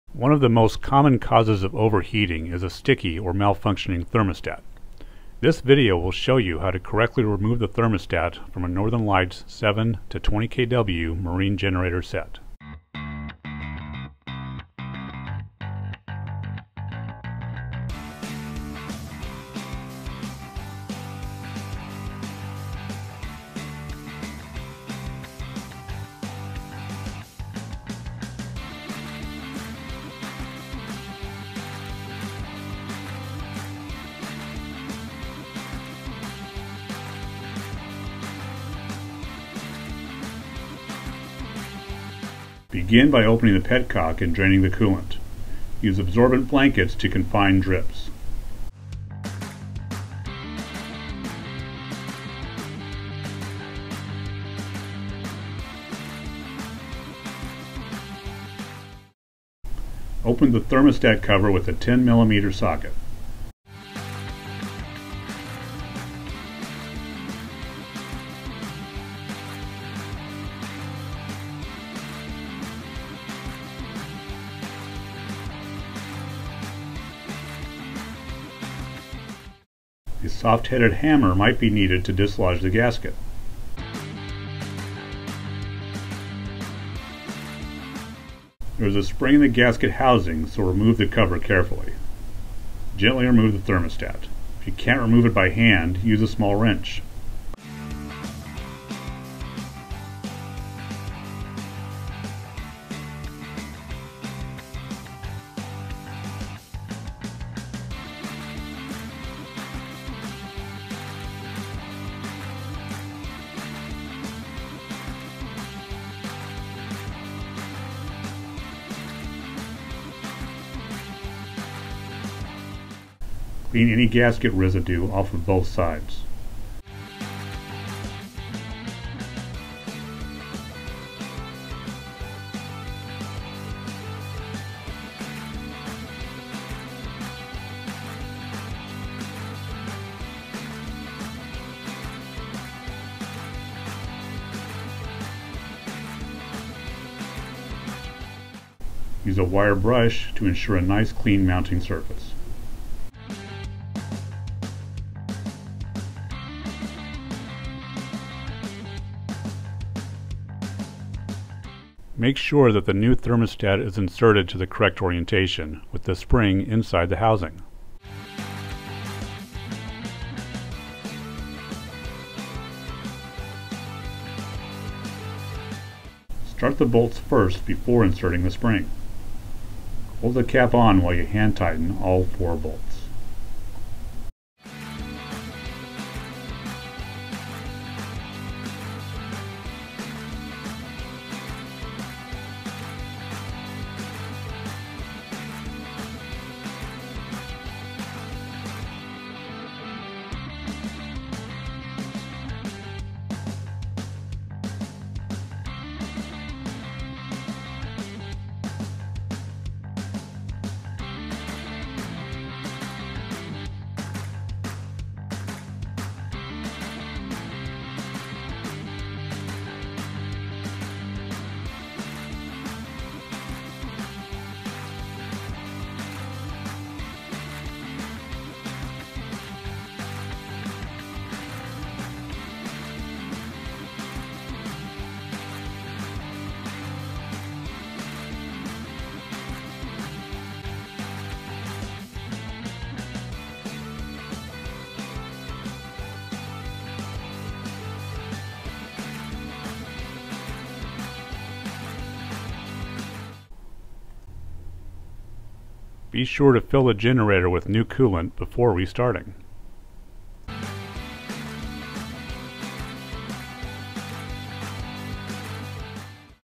Accessing & Replacing Thermostat on 7-20 kW Northern Lights Marine Generator
One of the most common causes of overheating is a sticky or malfunctioning thermostat. In this video we will demonstrate how to access and replace the thermostat on your Northern Lights 7-20 kW marine generator. This video includes the tools needed, how to properly access the thermostat, clean the gasket and replace your thermostat.

Tools needed:
10mm socket wrench
Gasket scrapers
Wire brush
Soft faced hammer
Pliers
Scotchbrite pad